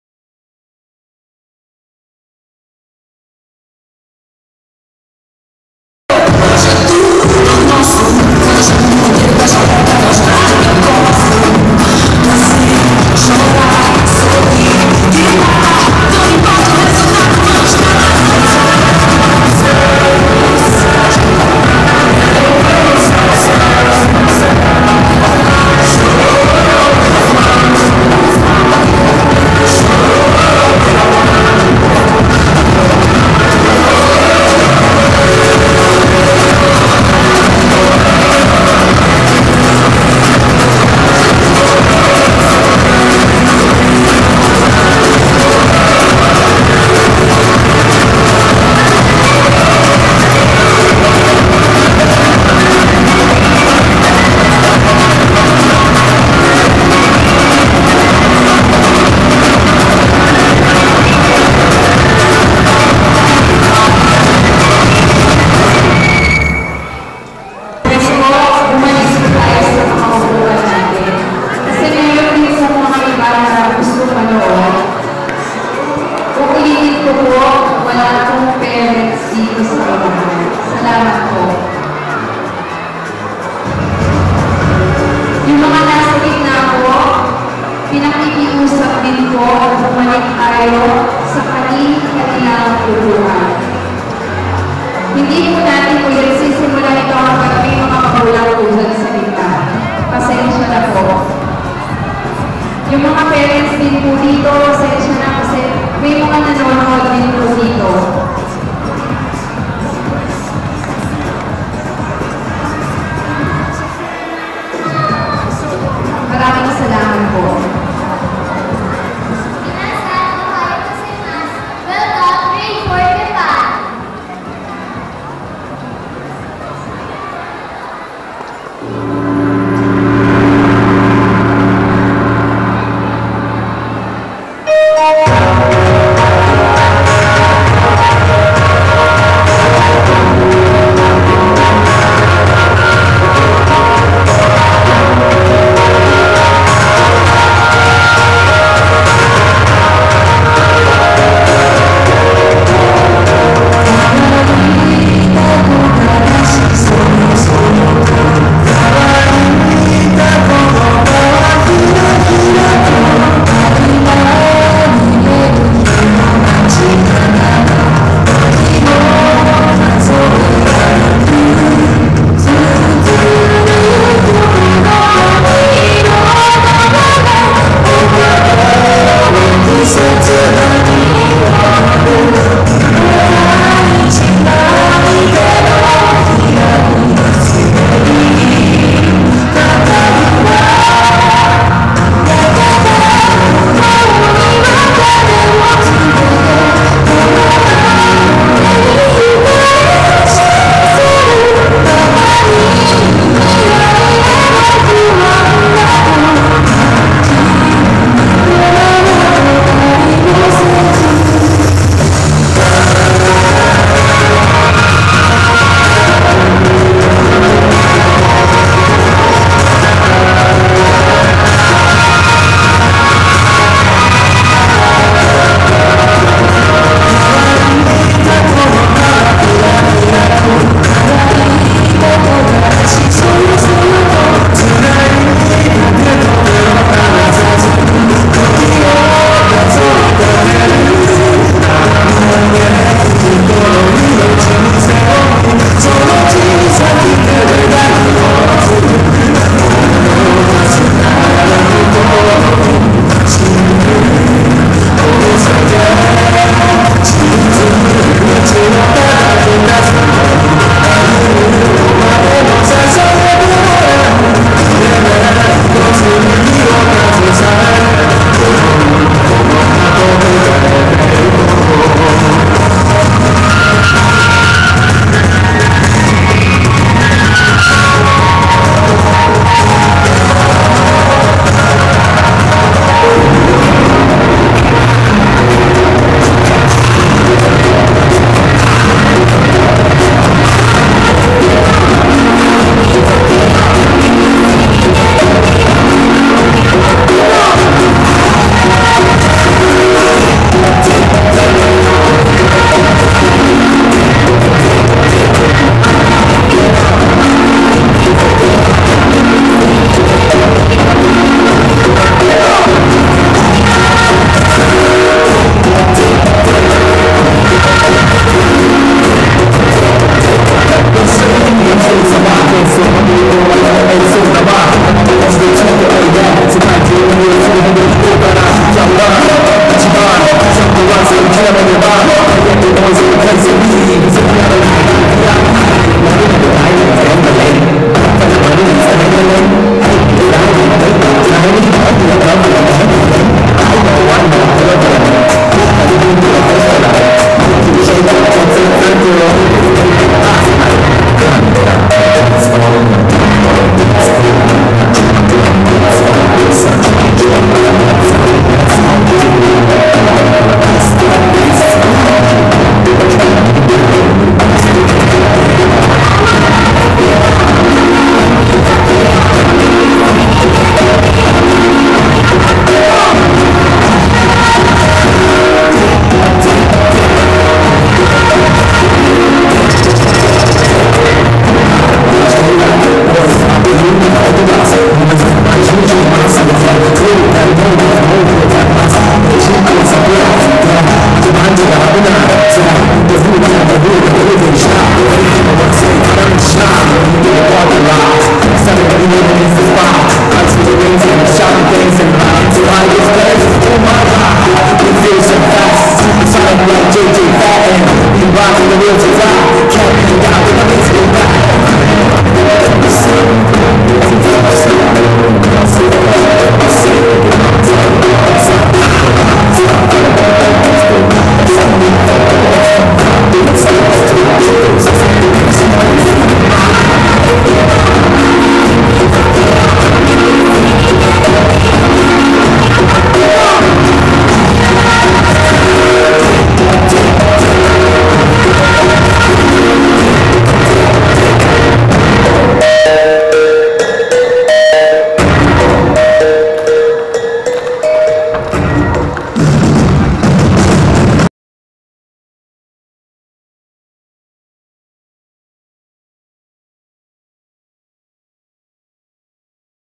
Chloe's Japanese Dance
Chloe's Grade IV class presents Japanese Dance to the 13th Commencement Exercises of the Holy Family Parochial School of Kapitolyo, Pasig City on March 19, 2016.